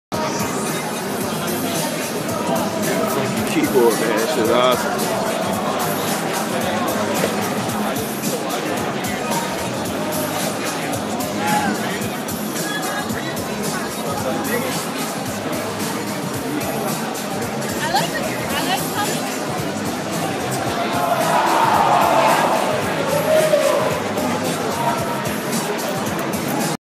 The Coolest Computer Keyboard We've Ever Seen! | TECH VLOG
New enhanced keyboards change colors based on your mood and conditions inside the computer. The keyboard controls can be re-configured for gaming, video editors, or programmers.

Digilyfe Magazine: News, Entertainment, and Lifestyle for the Digital Age.